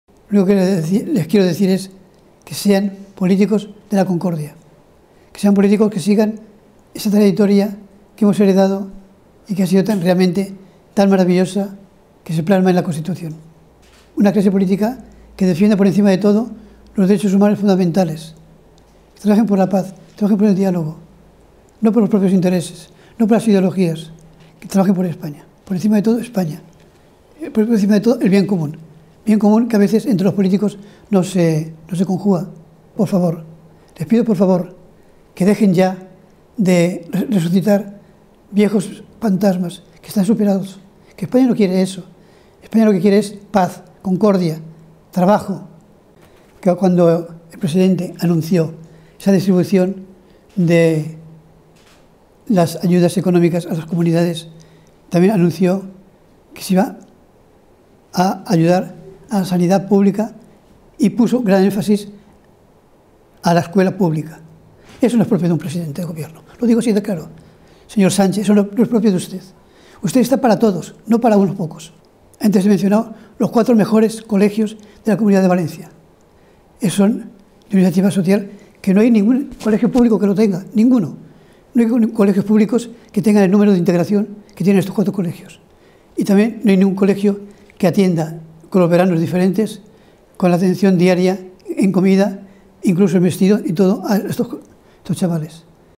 El Cardenal Cañizares pide “derechos inalienables para la Educación de iniciativa social”
El Cardenal Arzobispo de Valencia, Antonio Cañizares ha presentado esta mañana la Comisión creada por el Arzobispado ante las consecuencias económicas y sociales originadas por la pandemia del COVID-19, comprometida a colaborar con otras instituciones civiles y sociales, que atenderá prioritariamente el empleo digno y la regeneración social, junto a otros aspectos necesarios para el bien común de la persona y el tejido social.